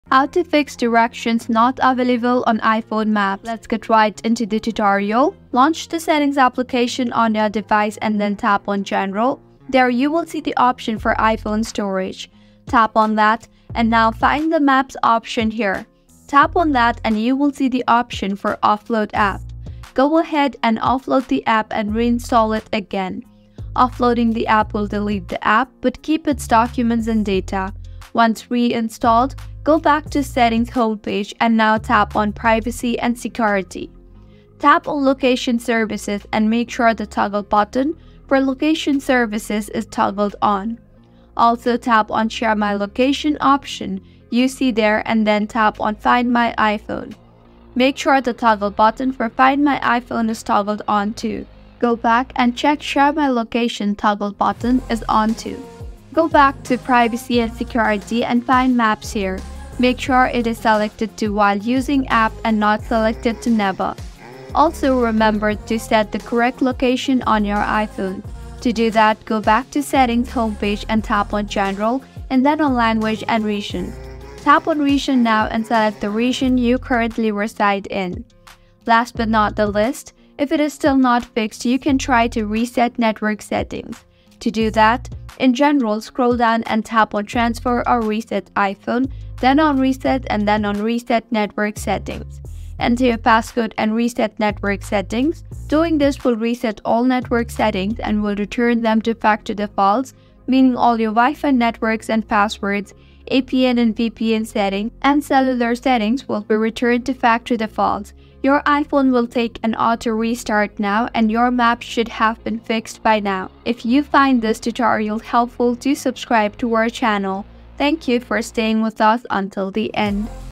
How To Fix Directions not Available on iPhone Maps | iPhone Map Direction Issues
👋 Greetings iPhone Explorers! Facing frustration when your trusty iPhone map fails to provide directions? Don't worry, we've got your back! Learn how to fix "Directions Not Available" on iPhone Maps and get back on track. In this guide, we'll troubleshoot map direction issues step by step, so you can navigate hassle-free.

🗺️ Navigating to a Solution: How to Fix Directions Not Available on iPhone Maps:

📌 Have you encountered this map issue before? Share your experiences and tips for solving map problems in the comments below.

📱 Let's get started:

0:00 - Welcome to the Map Direction Troubleshooting Guide!
0:05 - Step-by-Step: How to Fix Directions Not Available on iPhone Maps